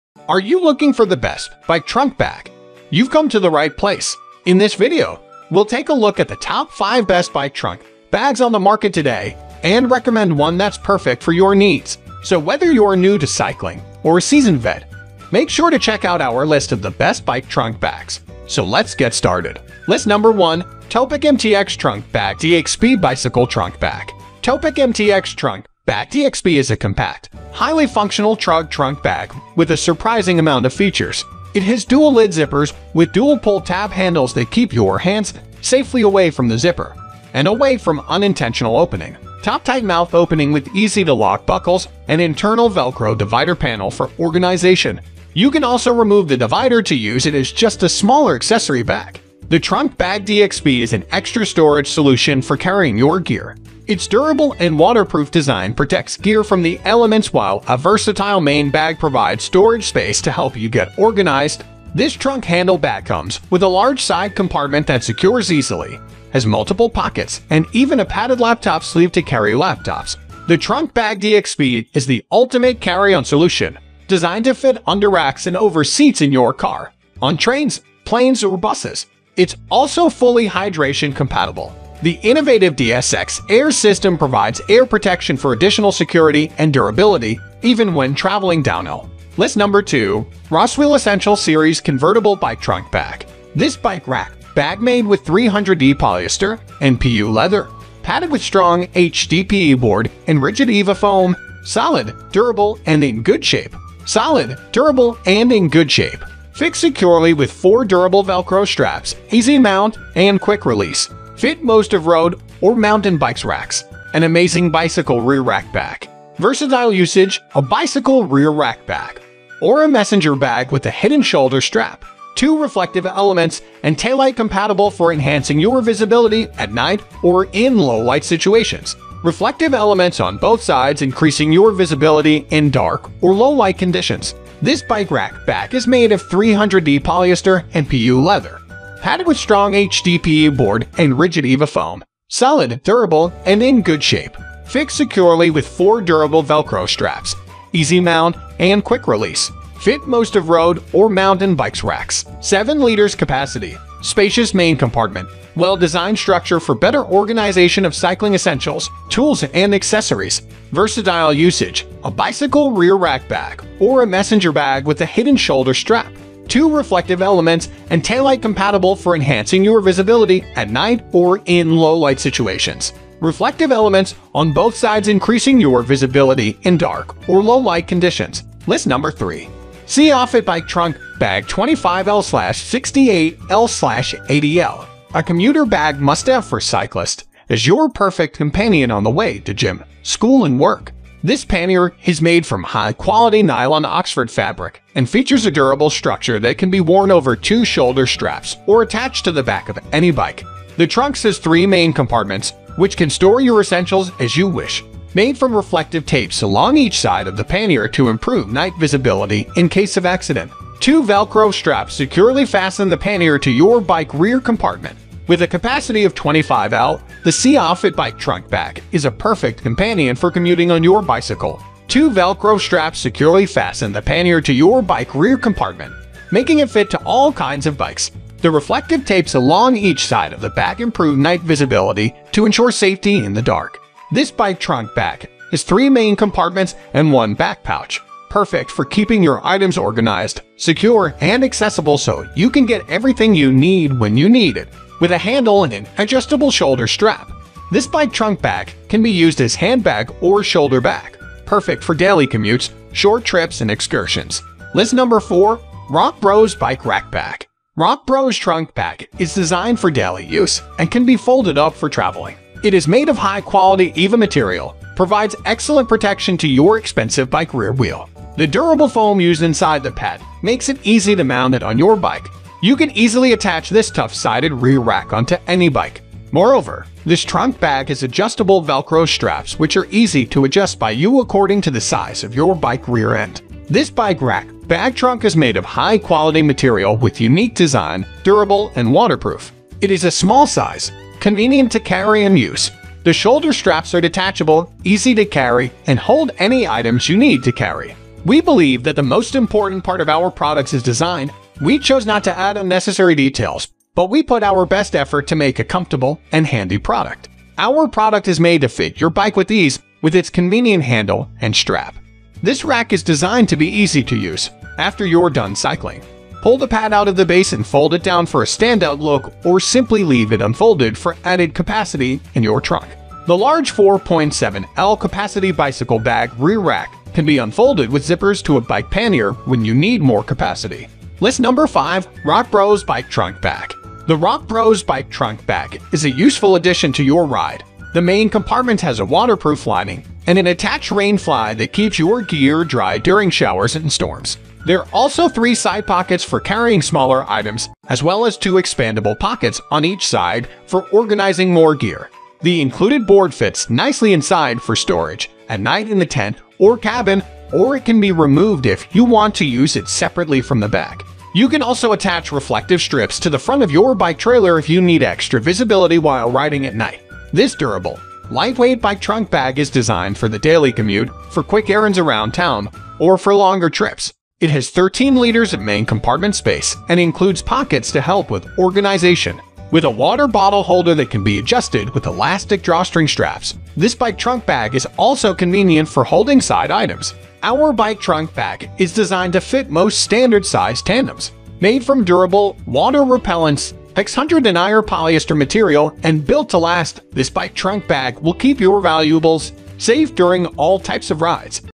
TOP 5 Best Bike Trunk Bag 2023
In this video, we have listed the top 5 best bike trunk bag on Amazon Reviews and buying Guide 2023

1. Topeak MTX Trunk Bag DXP Bicycle Trunk Bag with Rigid Molded Panels
2. Roswheel Essential Series Convertible Bike Trunk Bag/Pannier
3. COFIT Bike Trunk Bag 25L / 68L / 80L, Extendable Large Capacity Saddle Bags Waterproof Bicycle Rear Rack Luggage Carrier Perfect for Cycling, Traveling,
4. ROCKBROS Bike Rack Bag Trunk Bag Waterproof Carbon Leather Bicycle Rear Seat Cargo Bag Rear Pack Trunk Pannier Handbag
5. ROCKBROS Bike Trunk Bag Bicycle Rack Rear Carrier Bag Commuter Bike Luggage Bag Pannier eBike Accessories Storage Bags With Rain Cover

Are you  looking for the best bike trunk bag, you've come to the right place! In this video, we'll take a look at the top five best bike trunk bags on the market today, and recommend one that's perfect for your needs. So whether you're new to cycling or a seasoned vet, make sure to check out our list of the best bike trunk bags. so lets get started.